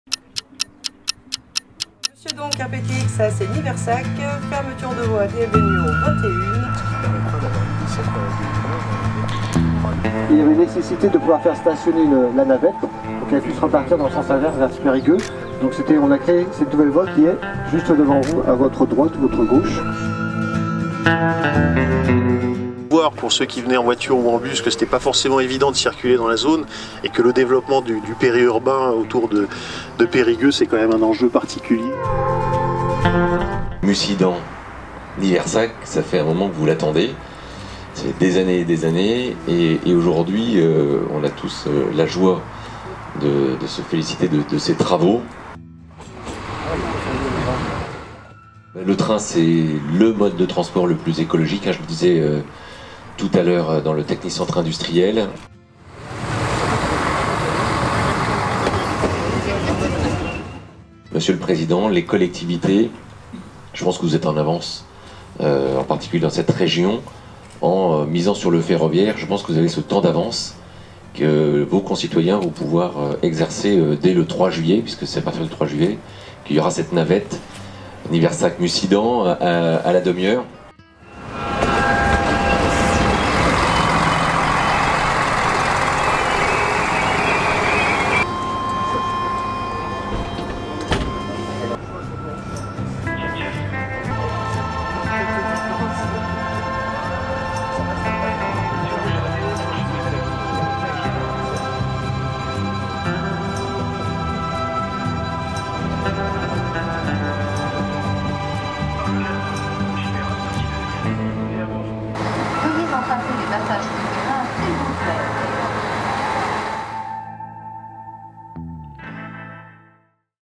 Dordogne : une navette ferroviaire Niversac-Mussidan pour cet été
Le 3 juillet 2022, les usagers pourront emprunter toutes les demi-heures une navette ferroviaire qui reliera Niversac à Mussidan. Mardi 1er mars, de nombreux officiels ont d'abord visité la gare de Niversac, où des gros travaux ont été effectués pour l'utilisation de cette navette, pour ensuite l'emprunter afin de se rendre sur le chantier de la gare de Périgueux. Vidéo Stéphane Klein